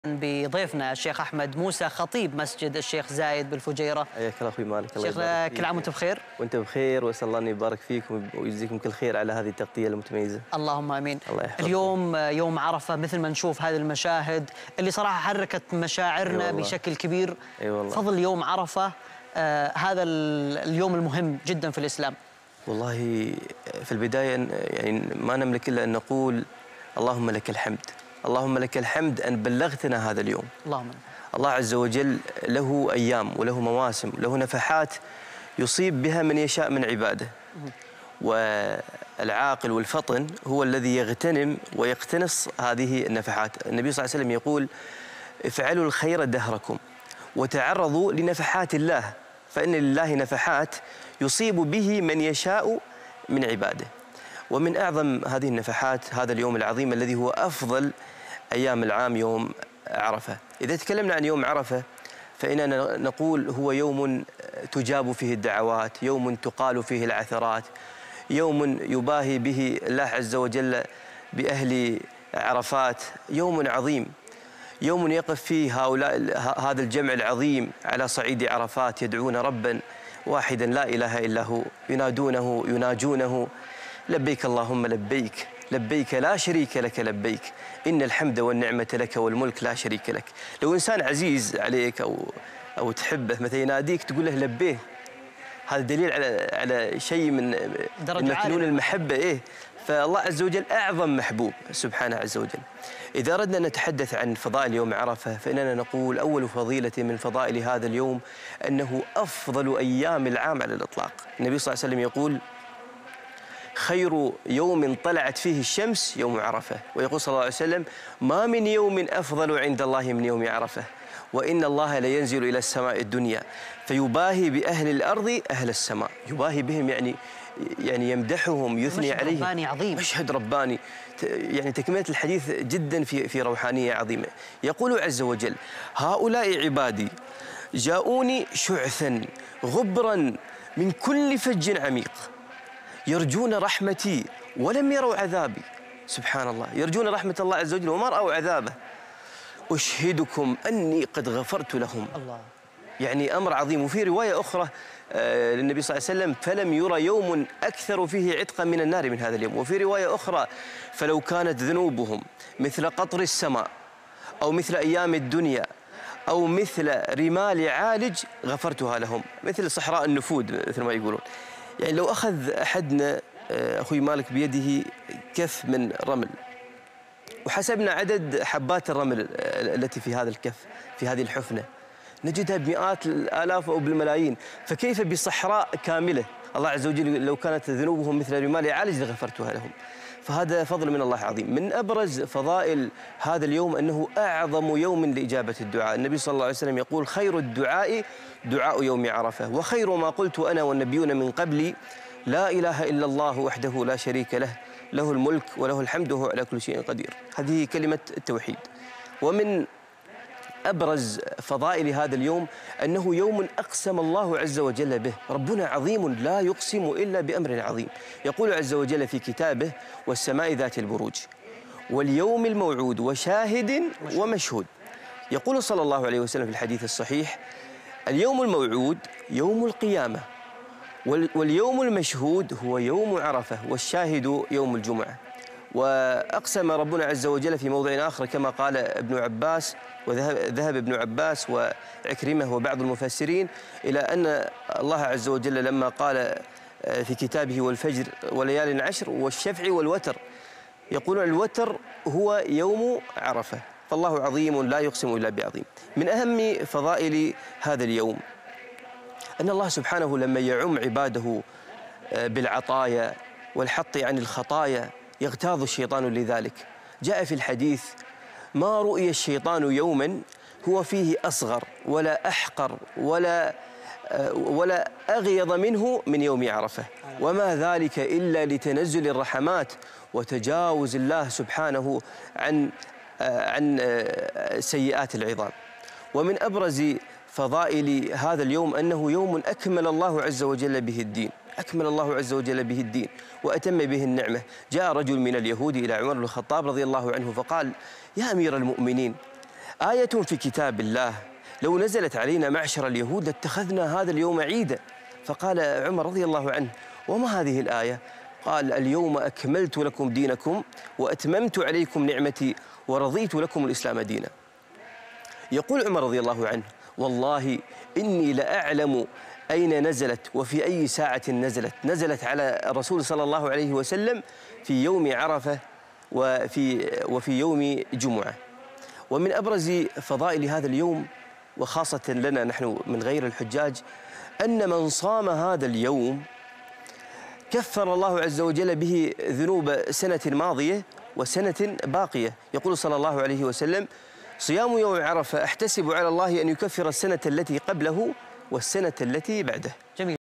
خطيب جامع الشيخ زايد بالفجيرة أحمد الموسى: هذا هو فضل صيام يوم عرفة لغير الحجاج
خطيب جامع الشيخ زايد بالفجيرة أحمد الموسى:
هذا هو فضل صيام يوم عرفة لغير الحجاج